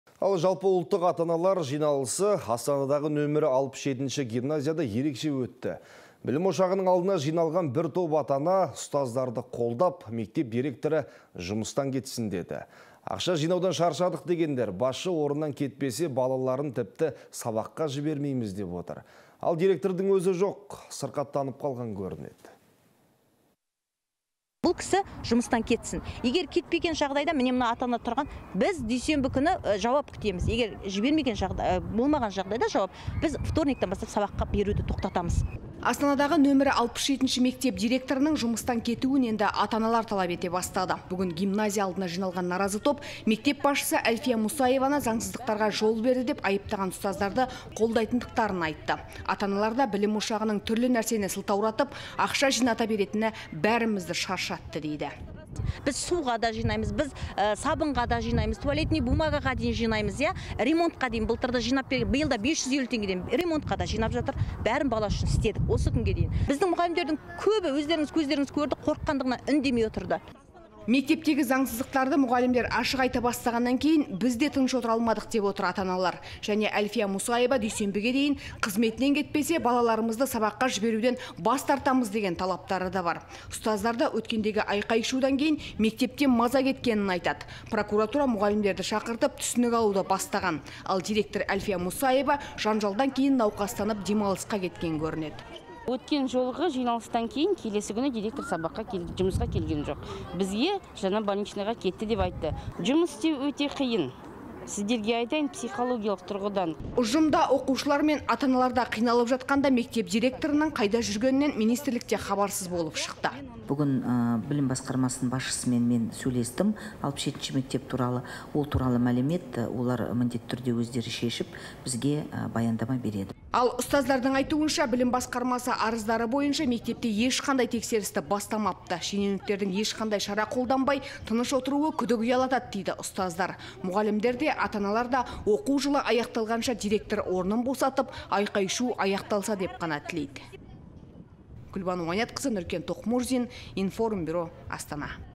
Бір топ ата-ана ұстаздарды қолдап, мектеп директорының жұмыстан кетуін талап етті
Жалпыұлттық ата-аналар жиналысы Астанадағы нөмірі 67 гимназияда ерекше өтті. Бүгін білім ошағының алдына жиналған бір топ ата-ана ұстаздарды қолдап, мектеп директорының жұмыстан кетуін талап етті. "Ақша жинаудан шаршадық" - дегендер басшы орнынан кетпесе, балаларымызды сабаққа жібермейміз деп отыр. Ал директор Әлфия Мұсаеваның өзі сырқаттанып қалған көрінеді.